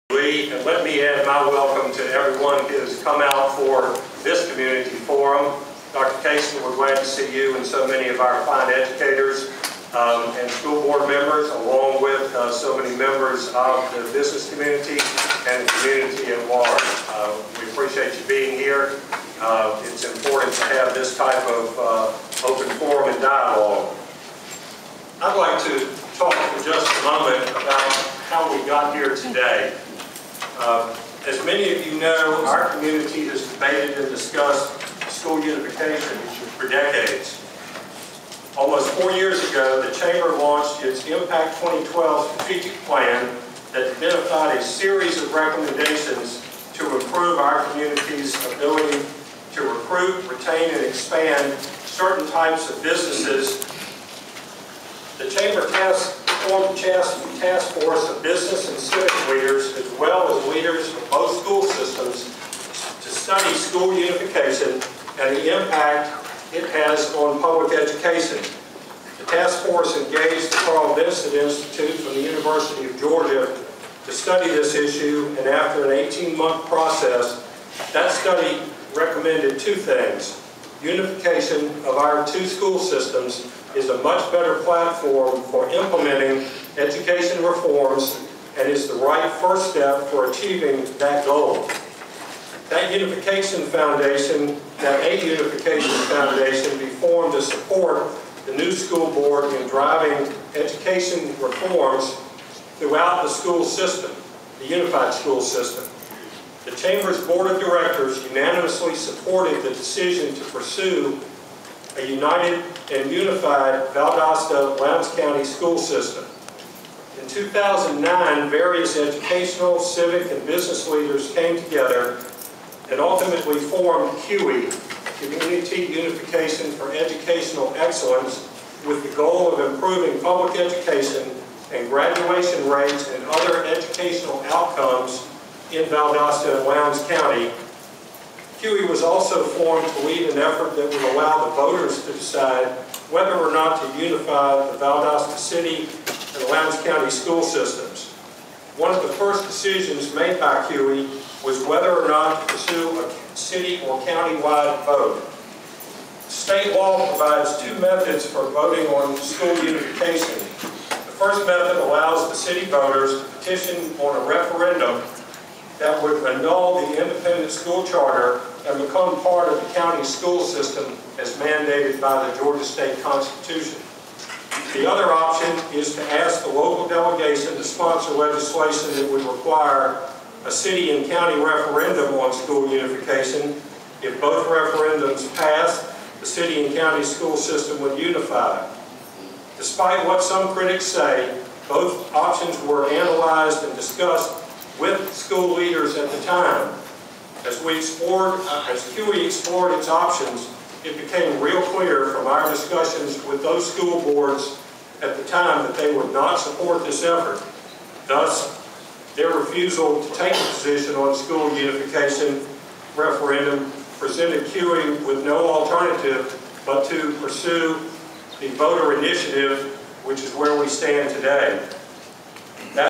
How we got here today --Tom Gooding @ CUEE 27 Sep 2011
School Unification
Forum, Community Unification for Educational Excellence, Inc. (CUEE),